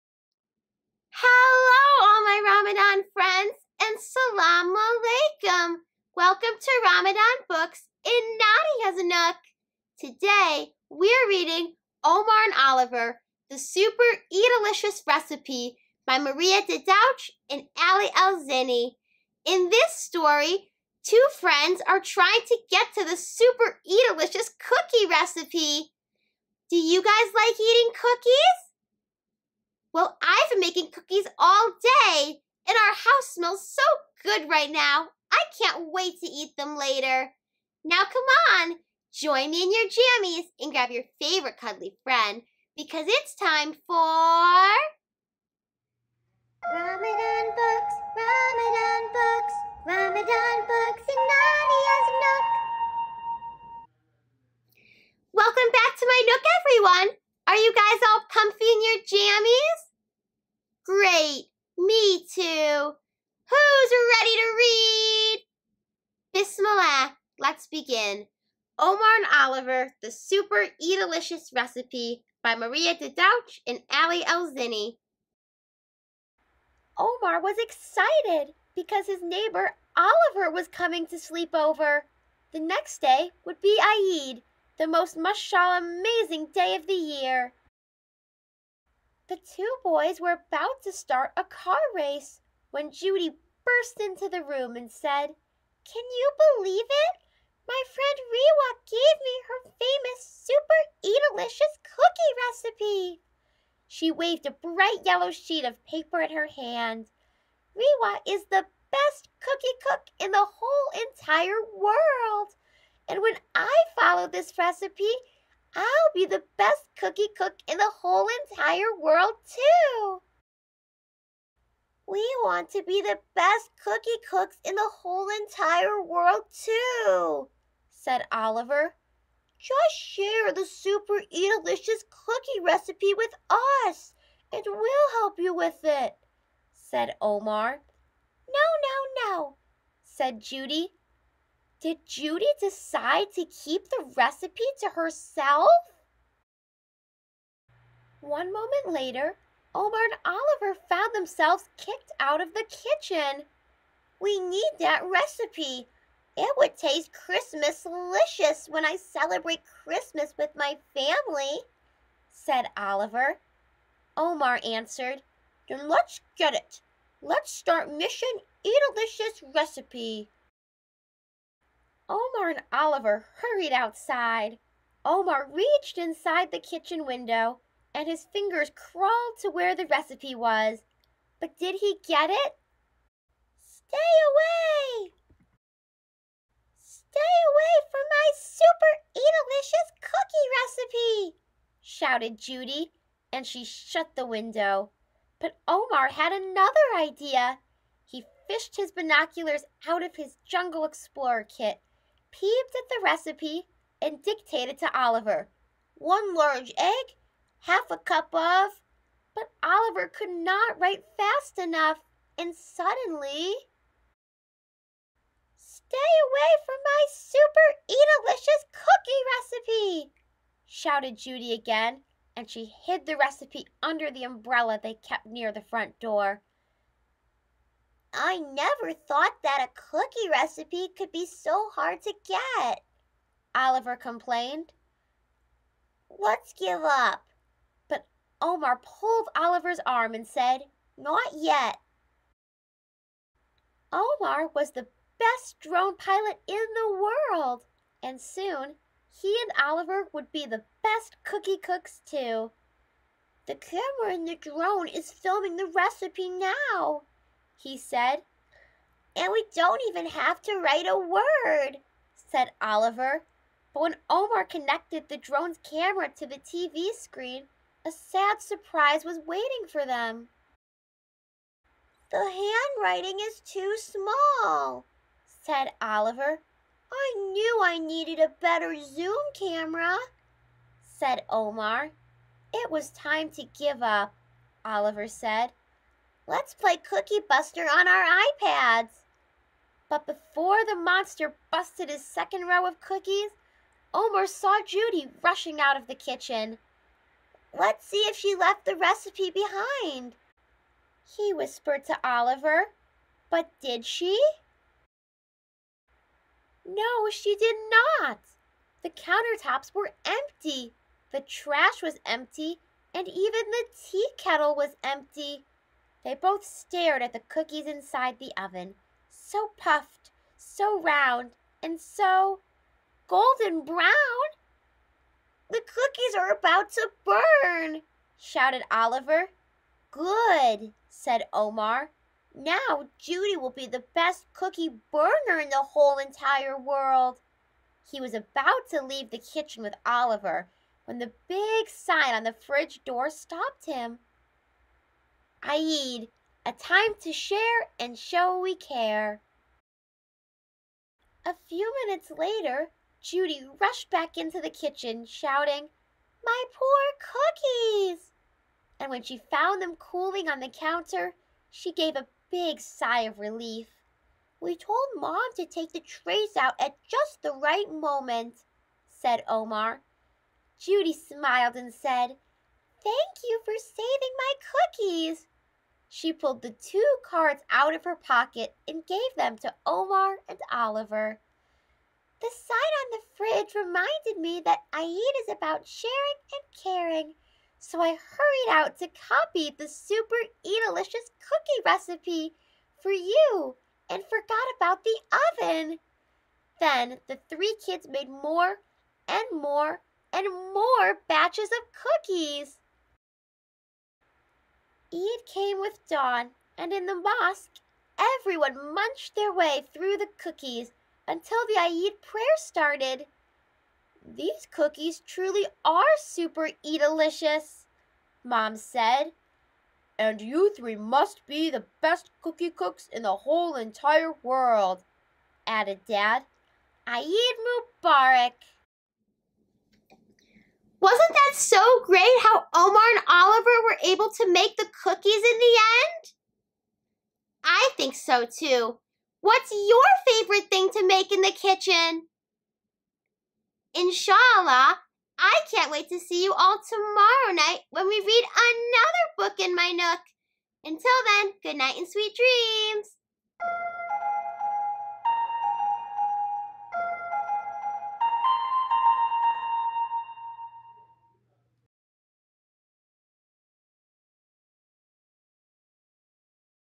Omar & Oliver Super Eidilicious Recipe
Welcome back to my nook everyone! Tonight we are reading, "Omar & Oliver's Super Eidilicious Recipe" by Maria Dadouch. So, join me in your jammies with your favorite cuddly friend...